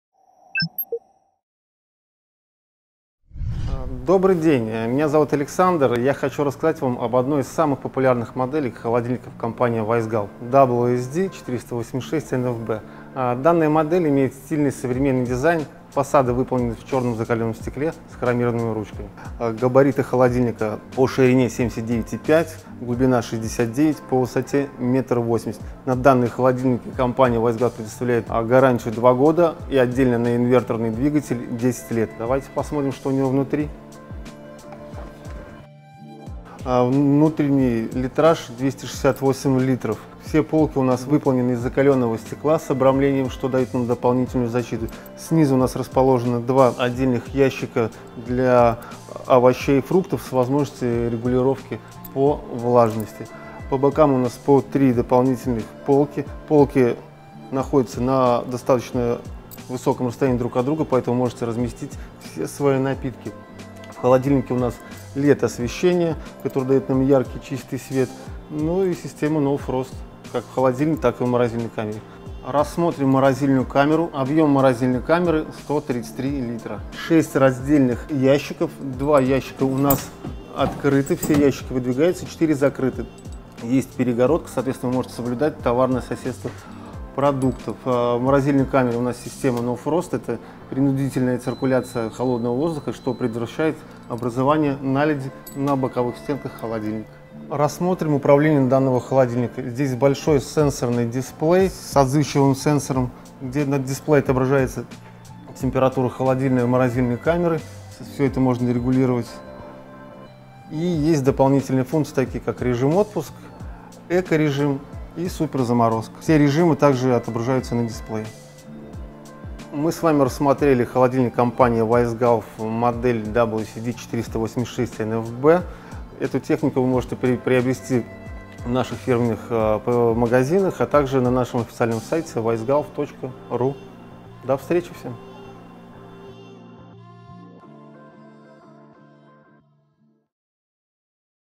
Функциональность на высшем уровне - Weissgauff WCD 486 NFB
Холодильник Weissgauff WCD 486 NFB - функциональность на высшем уровне.
ВМЕСТИТЕЛЬНЫЙ. Холодильная камера на 268 л, морозилка на 133 л. Габариты устройства 79,5 х 69,2 х 180 см. Идеально для тех, кто хотел холодильник побольше — легко разместите все необходимые продукты.
УДОБНЫЙ. Особая эргономика полок и 4-дверное открывание. Двойной спецконтейнер, сохраняющий свежесть овощей и фруктов.
ЭФФЕКТНЫЙ. Стильный фасад из черного закаленного стекла с крупным цифровым дисплеем — настоящее украшение вашей кухни.
ГАРАНТИЯ. Инверторный двигатель с 10-летней гарантией от Weissgauff. Меньше энергопотребление и уровень шума, больше надежность и срок службы вашего холодильника.